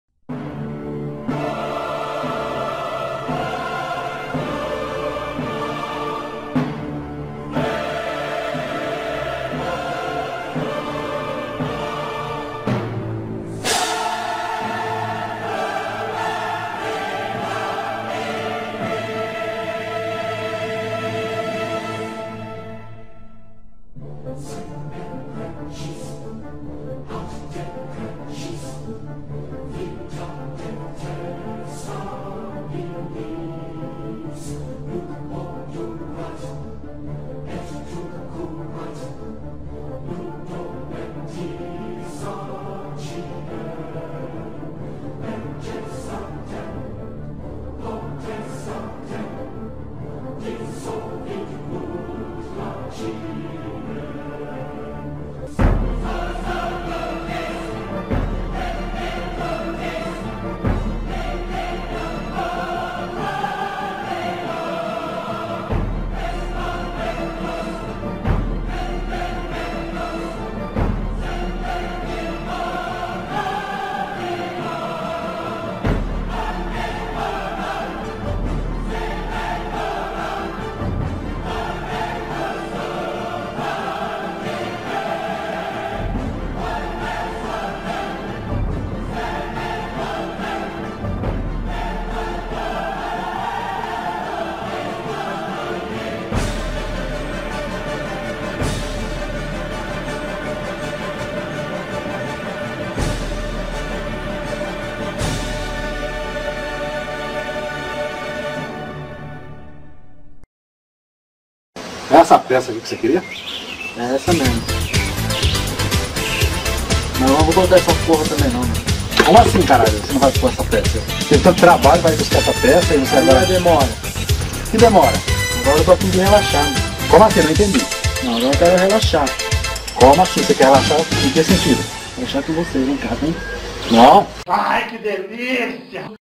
The Dawn of Jailson Trailer
O filme do ano esta para vir, Demacol o único sobrevivente não infectado pelo vírus do suco de laranja precisa sobreviver num mundo PÓS-APOCALIPSE. Com suas habilidades de pedreiro (Mason) será que ele irá conseguir construir um forte grande gostoso e firme para que ninguém deixe o oco nele?